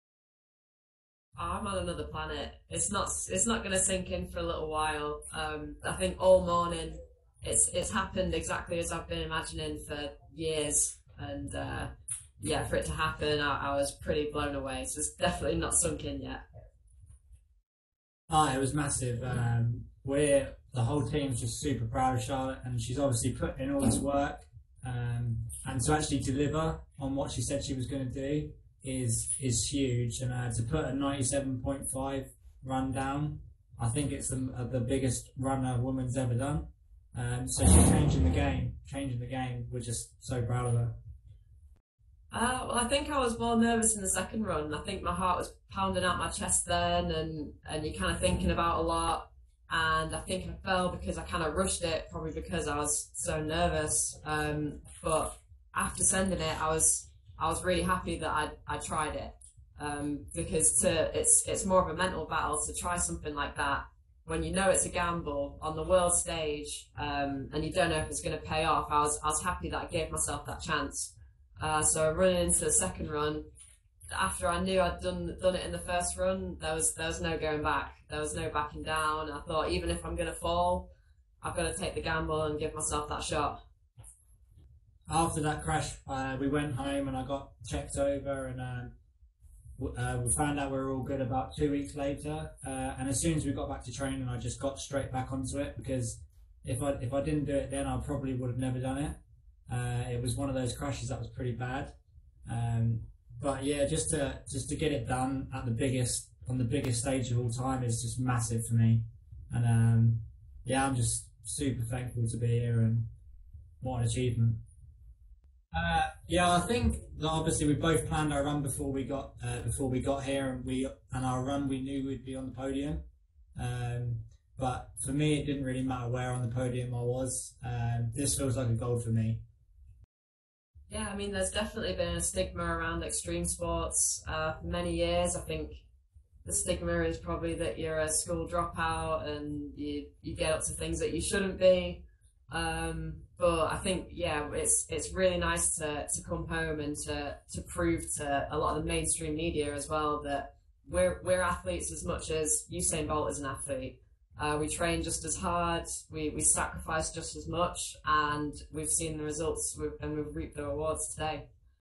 ‘On another planet’: British BMX medallists reflect on success at Tokyo Olympics
Charlotte Worthington and Declan Brooks speak about their achievements in the Tokyo 2020 Olympics.

Worthington made history twice over to become the first Olympic women’s freestyle champion.

The 25-year-old was the first woman to land a 360 back-flip in competition, part of a breathtaking routine that earned a mark of 97.50 and gold in a discipline making its Olympic debut.

In June, Brooks almost cost himself a place at these Games when he knocked himself unconscious attempting a double back-flip at the world championships in Montpelier, but he made the move the centrepiece of a routine in Tokyo which earned a score of 90.80 and bronze.